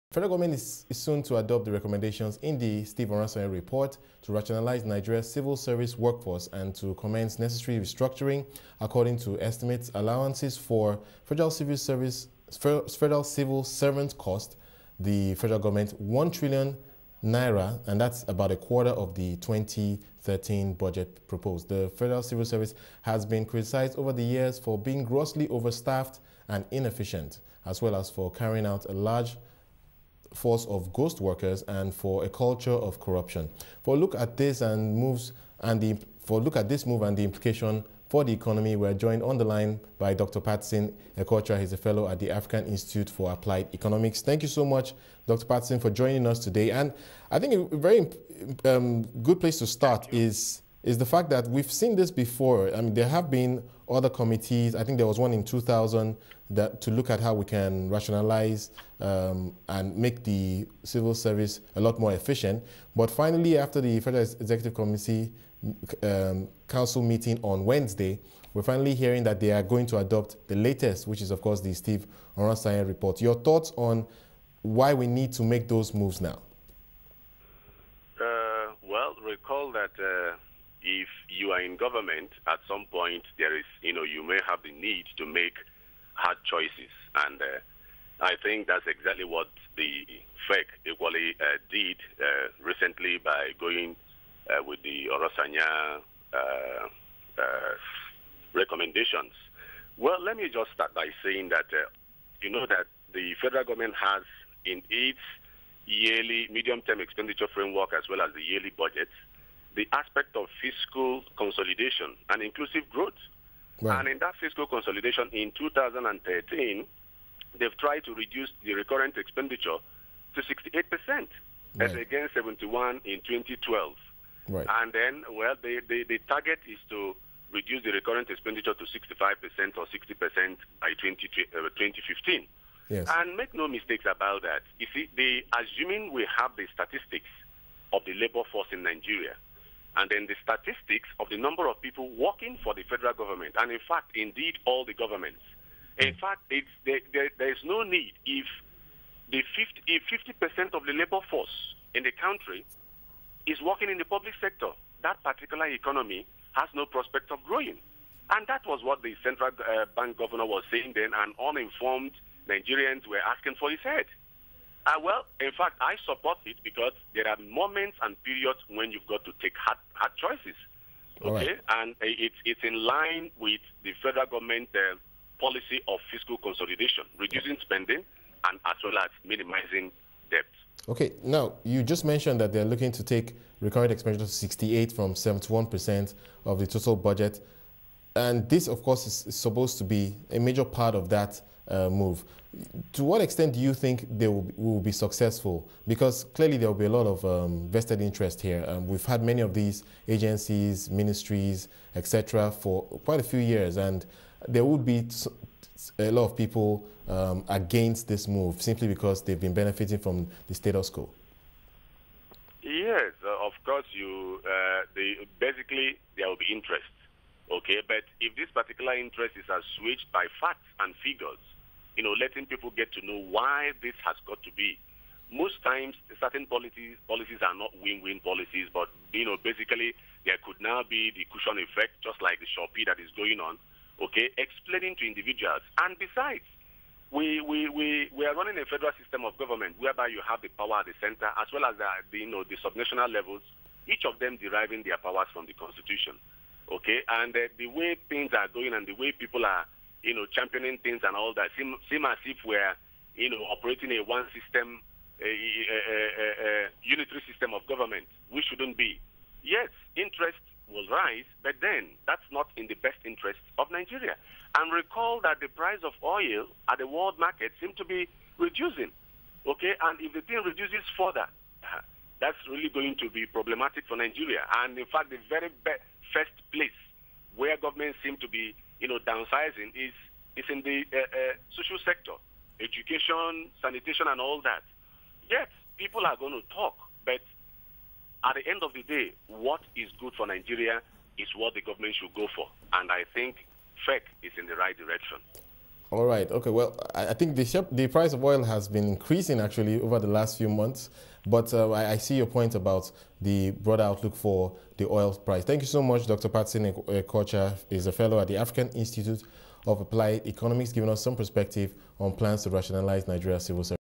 Plans to Rationalise Nigeria's Civil Service Workforce
The Federal Government will soon adopt a plan to rationalise Nigeria Civil Service workforce starting with the necessary restructuring. The federal civil service has been criticised over the years for being grossly over-staffed and inefficient.